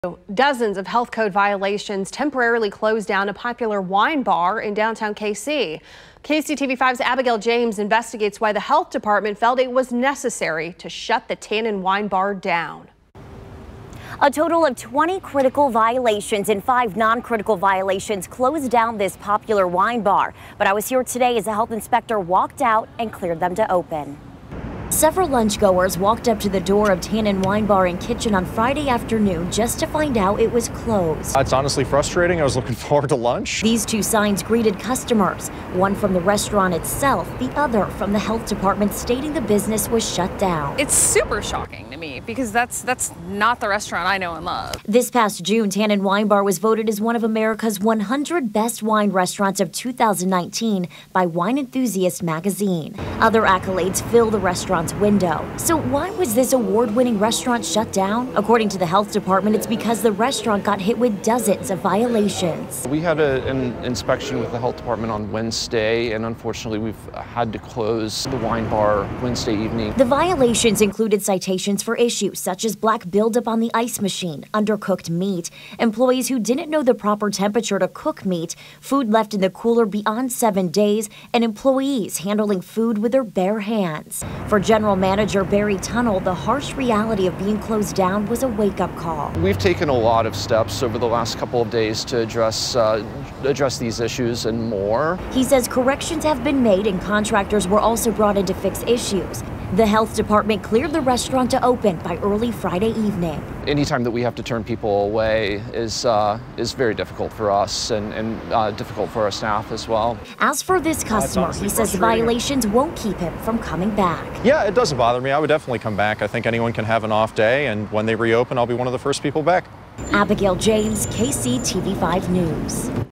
20 food safety violations temporarily close popular downtown KC restaurant
Kansas City health officials shut down Tannin Wine Bar and Kitchen downtown after the popular restaurant spot failed two food safety inspections in one day and was slapped with 20 critical violations, city records show.